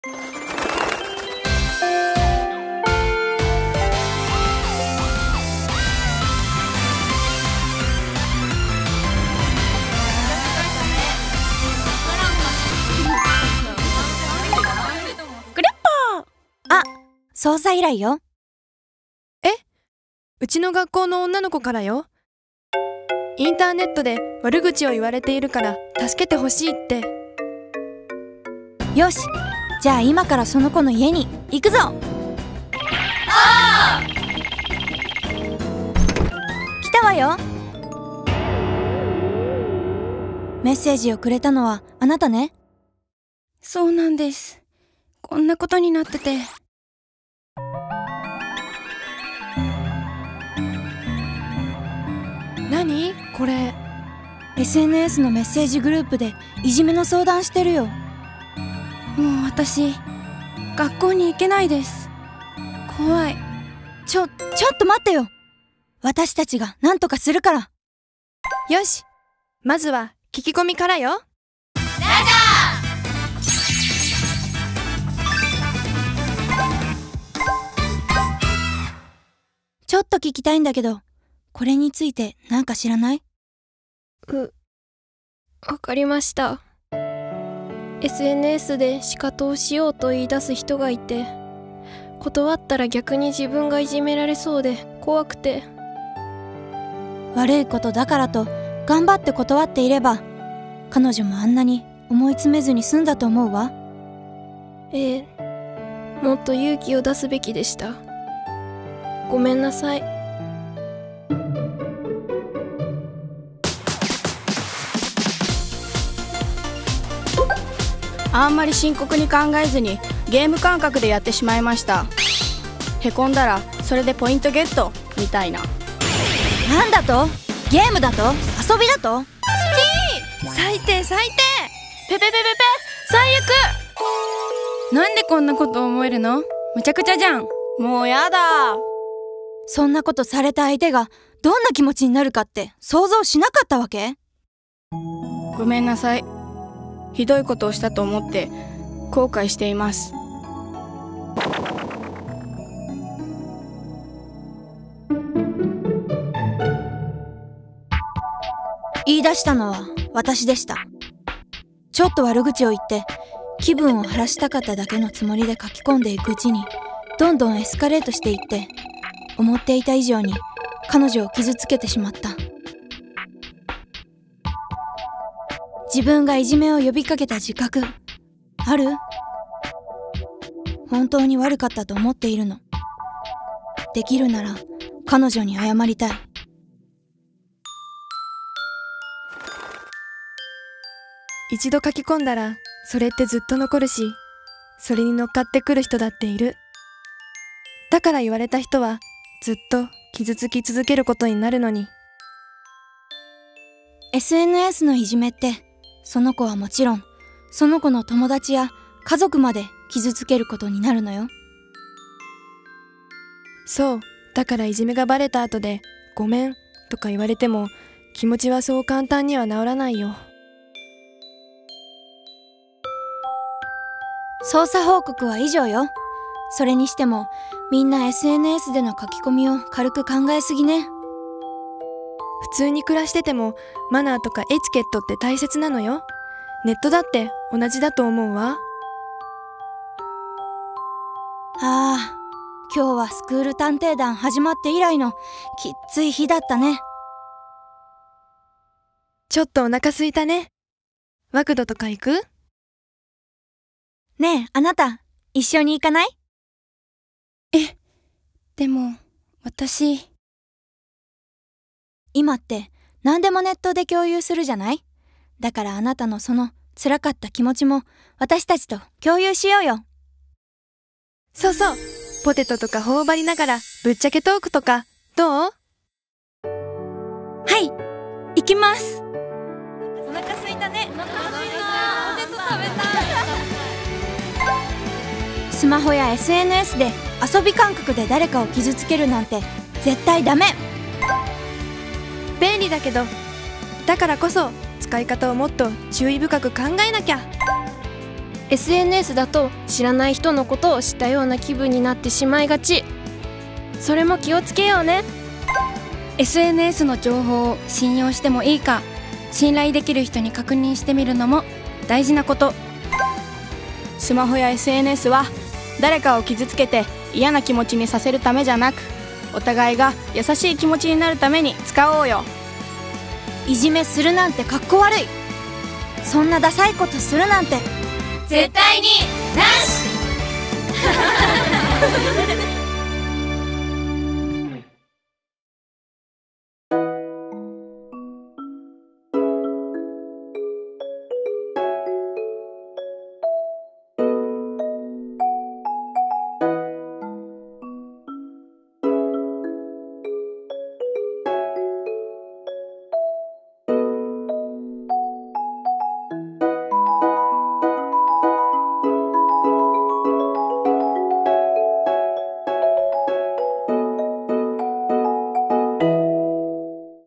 スクール探偵団「ふぃーるねおねお」～スマホ・ネット適正利用啓発動画
岡山県内の学校に通うスクール探偵団「ふぃーるねおねお」が、ＳＮＳ利用によるいじめの調査依頼を受け、その経緯や問題点を追求する動画です。
ＳＮＳ利用の注意点について、スクール探偵団「ふぃーるねおねお」と一緒に考えましょう。

▼専用ＬＰ「気を付けよう！スマホ・SNSの使い方」はこちら

▼県ＨＰ「ケータイ・スマホの正しい使い方」はこちら

【制作】
県民生活部男女共同参画青少年課